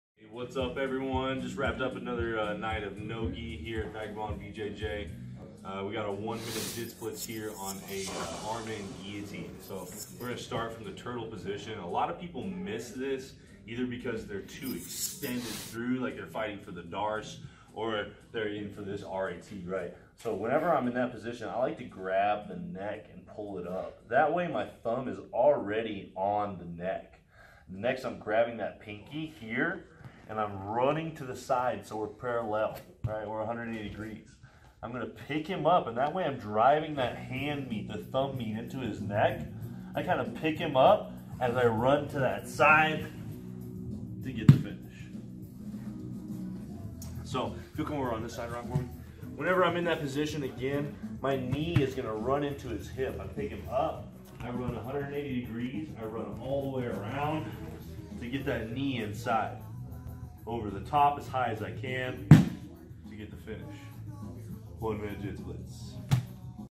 Jitz Blitz #12
Today we go over an arm in guillotine detail that will have your finish percentage go waaaaaaay up!